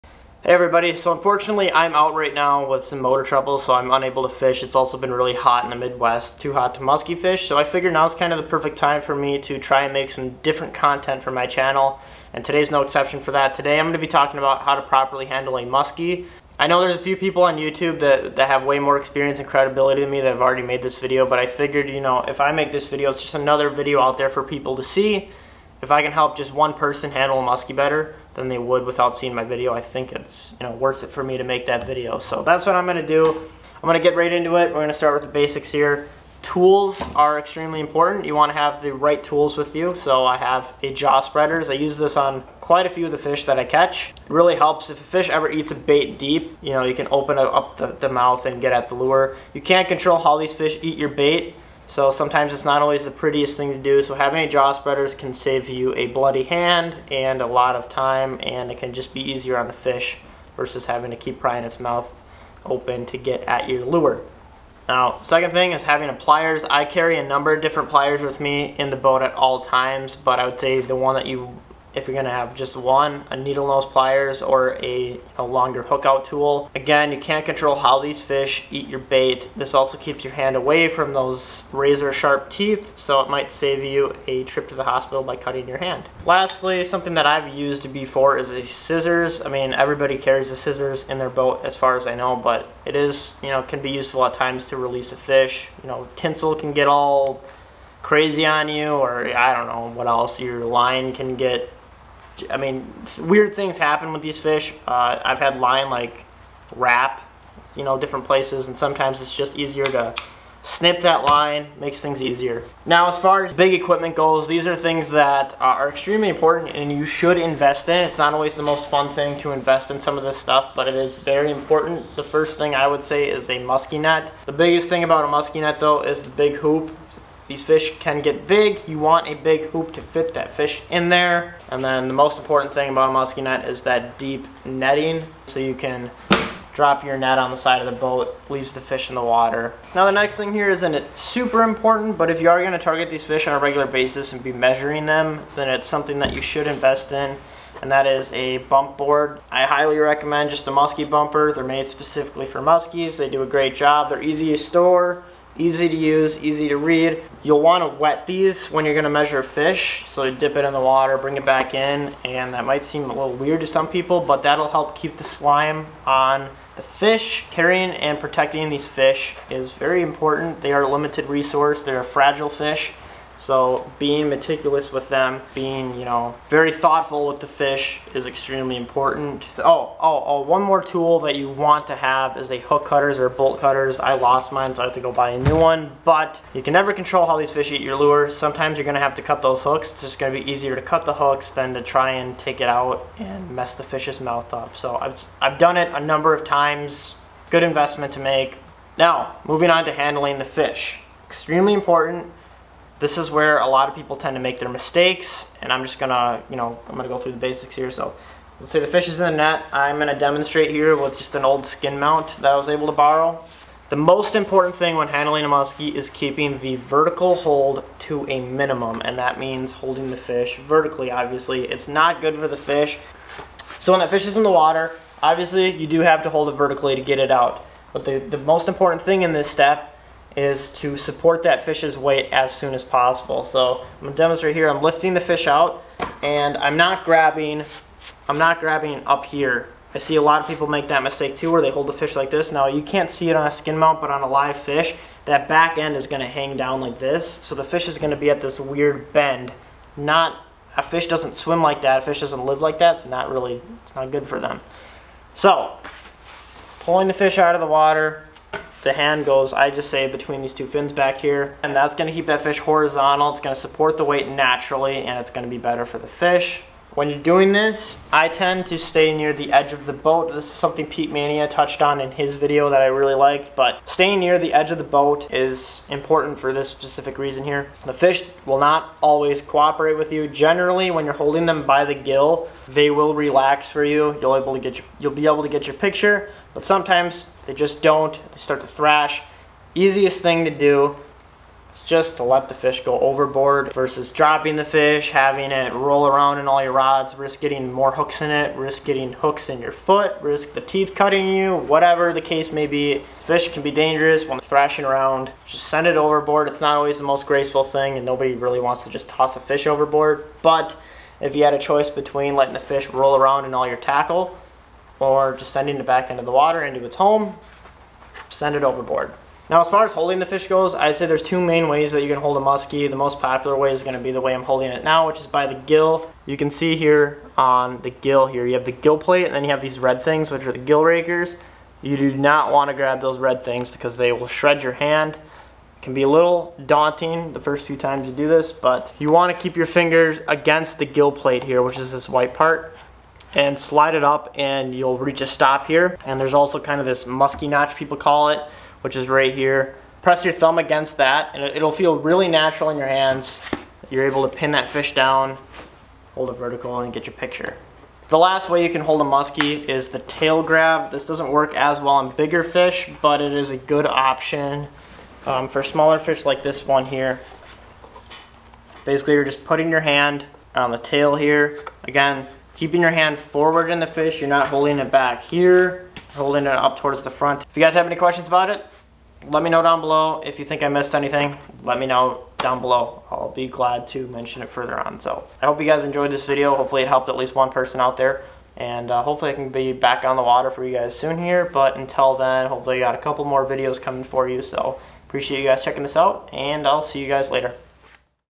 How You Should Try To Handle A Musky
Muskie are a fragile fish just as much as they are a limited fish. Properly handling these fish can make or break a fish becoming and or staying a trophy fish. I care deeply about how these fish are treated and I hope to pass on some good advice to new comers.